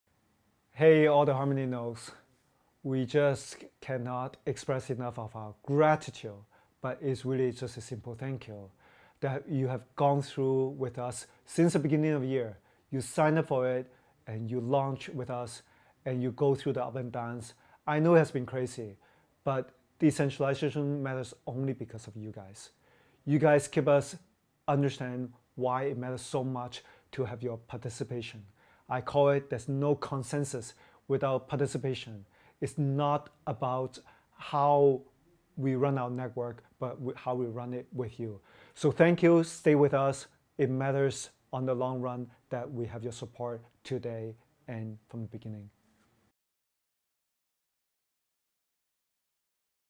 Stephen Tse: a big thank you to Node Runners.
Nodes are essential to the success of any decentralised permissionless public blockchain. To learn more about how you can get involved running a node on Harmony and contributing to a truly decentralised economy join our Telegram or Discord channels.